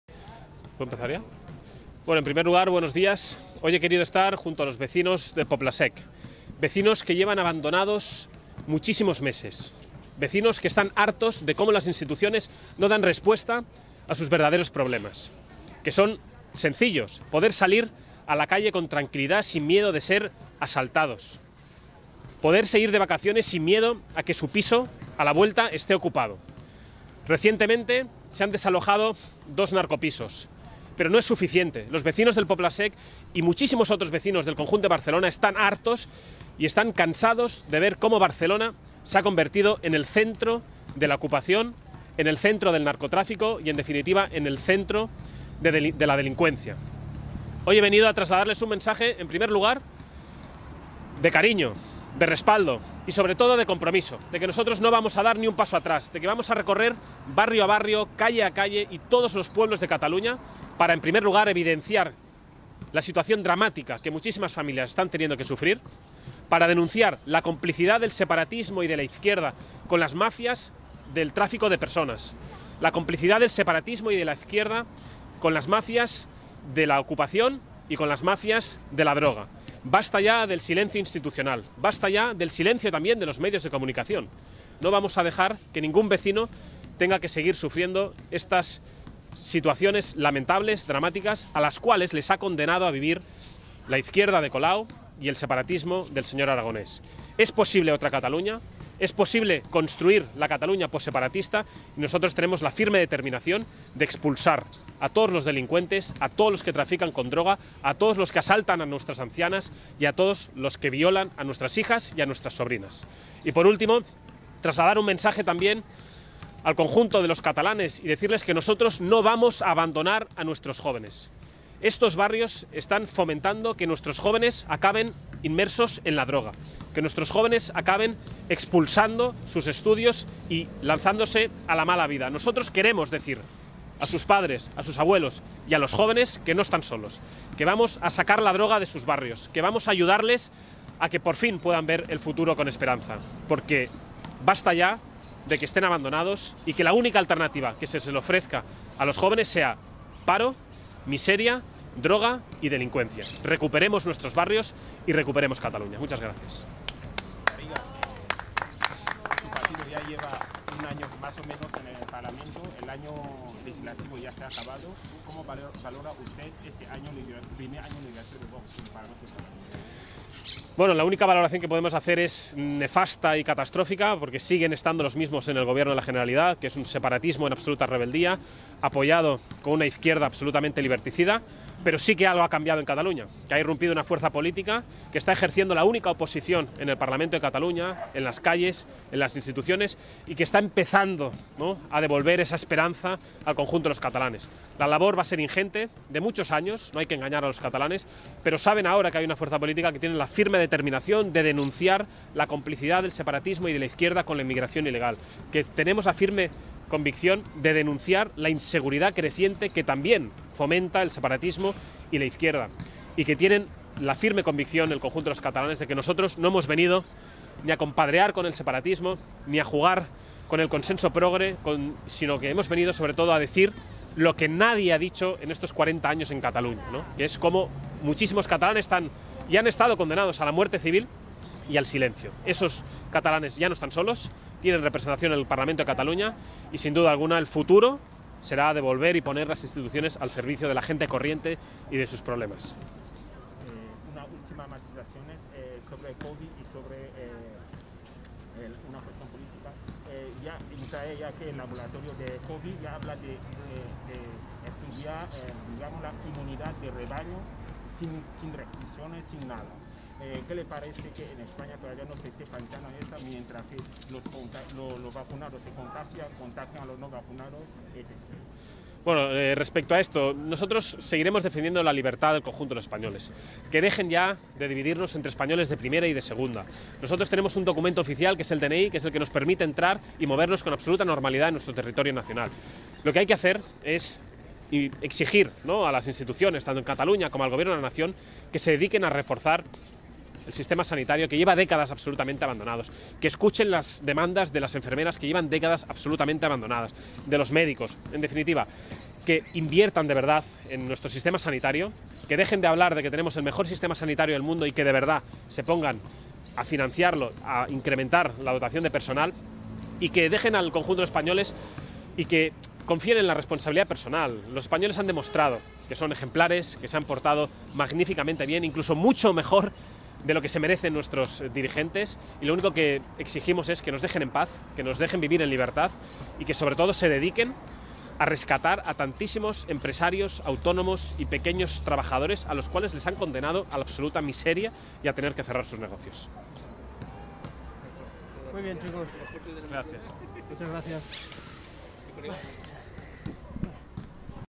Garriga VOX visita Poble Sec, la barriada de 'narcopisos' de la ciudad de Barcelona
Barcelona (España), viernes 31 de diciembre de 2021. "Es posible construir otra Cataluña, la Cataluña postseparatista". VOX tiene "la firme convicción de expulsar a todos los delincuentes, a todos los que trafican con droga, a todos los que asaltan a nuestras ancianas y a todos los que violan a nuestras hijas y sobrinas". Así ha manifestado el presidente del Grupo Parlamentario de VOX en el Parlamento de Cataluña, Ignacio Garriga Vaz de Concicao, esta mañana en declaración a la prensa desde el barrio de Poble Sec de la ciudad de Barcelona, azotado por narcotráfico.